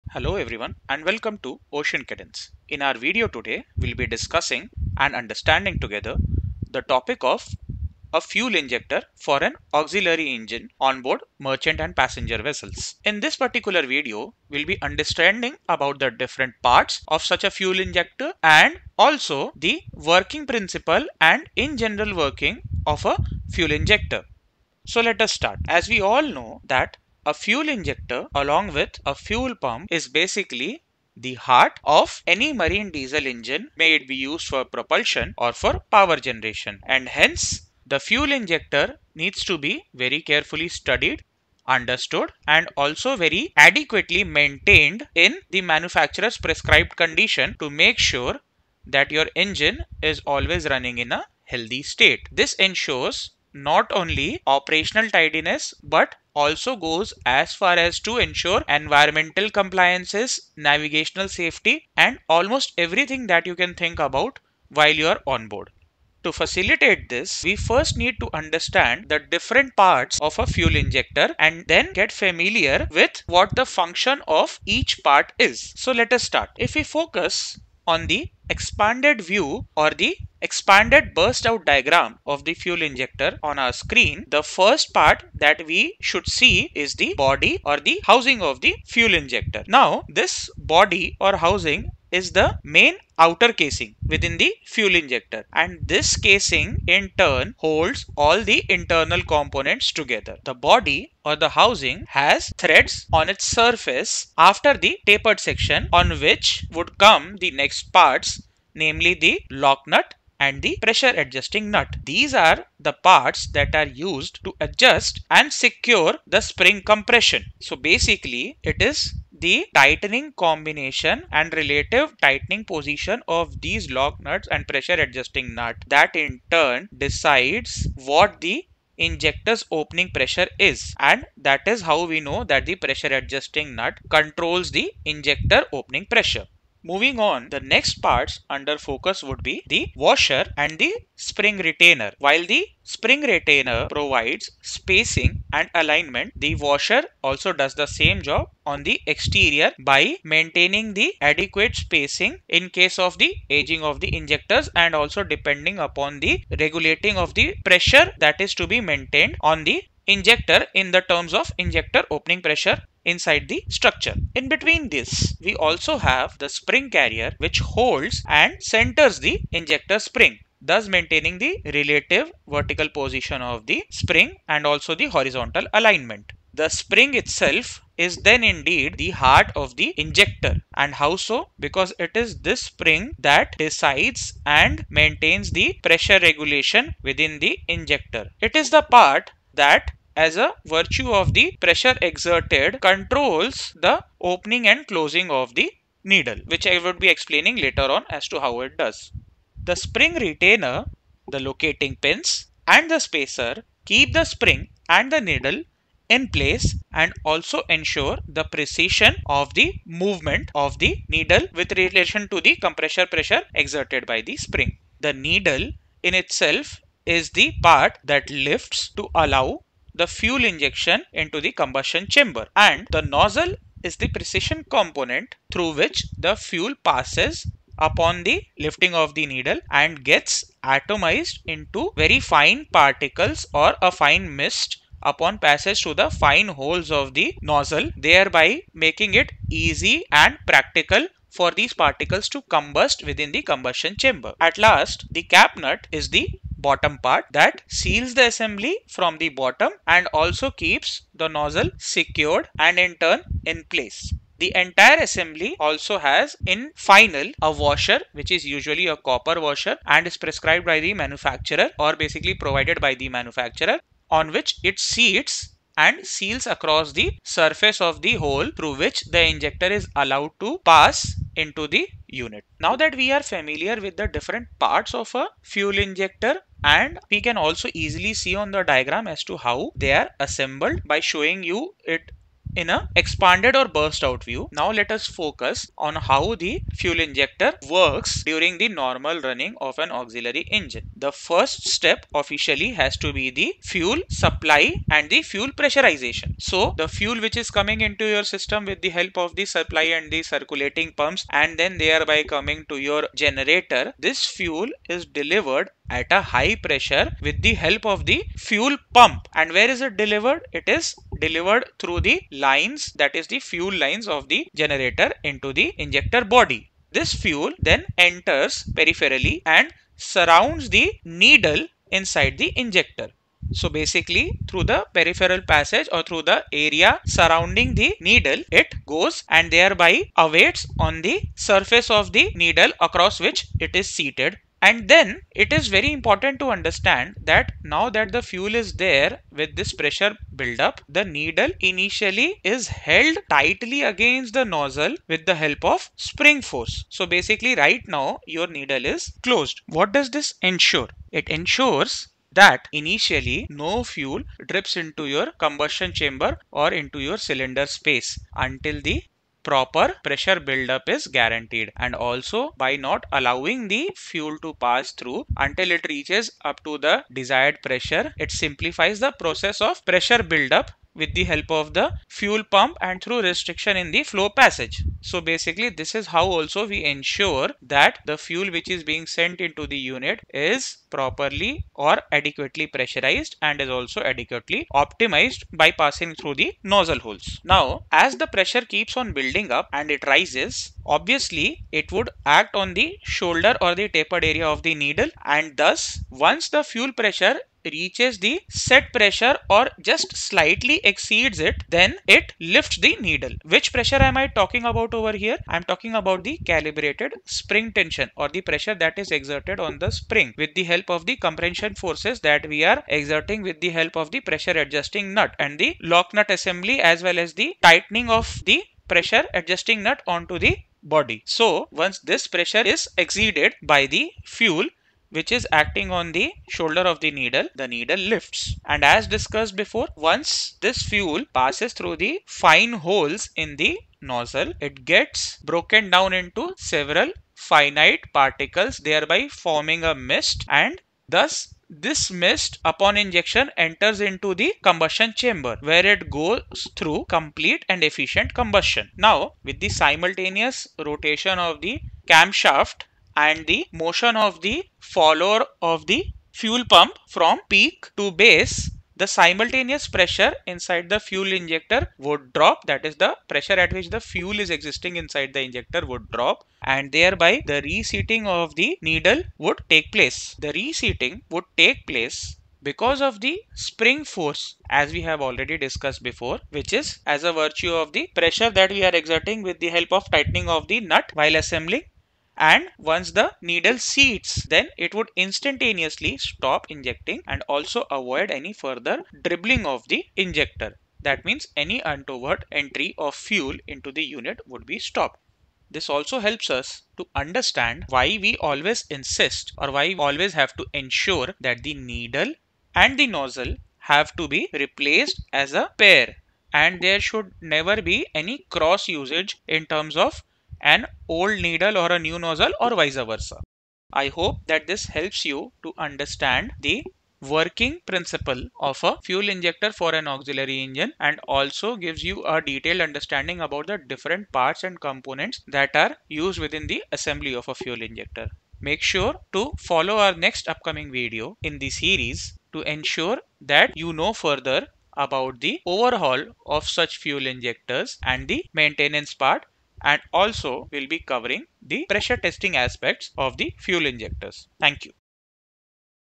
Auxiliary Engine Fuel Injector Working Animation| AE Fuel Injector Parts | Generator Fuel Injector
Good day viewers!

Our latest videos deals around the topic of a fuel injector used onboard ships for AUXILIARY ENGINE, with all the components and the working principle.

In the video, we have explained:
How an AE FUEL INJECTOR works
Components of a Fuel Injector
Working of each component of Fuel Injector
Fuel Injector Pressure Adjustment